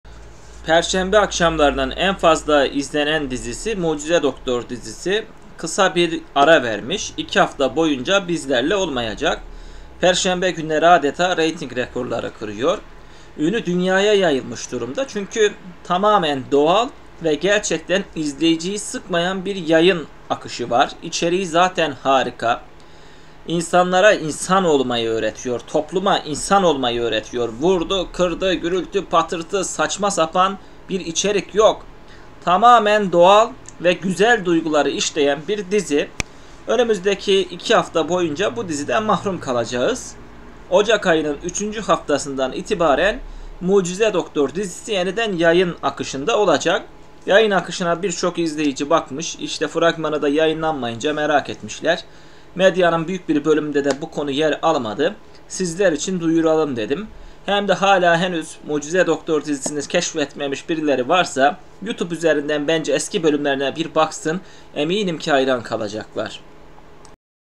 Mucize Doktor yayın akışında neden yok?
Mucize Doktor neden ara veriyor.